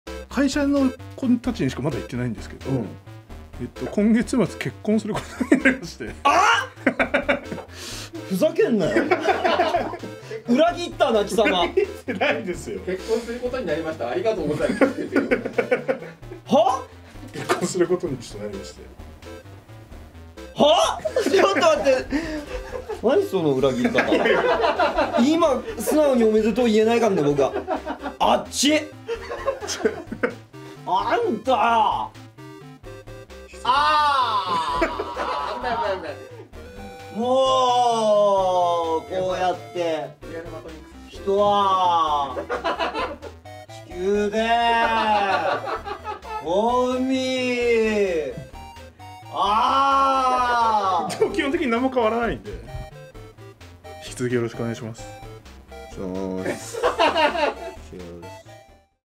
【はぁ？】元ラジ休憩中 番外編
元ラジ収録の一コマをお届けします。
10月のDX生配信後の出来事。引き続き、チーム元ラジをよろしくお願いいたします。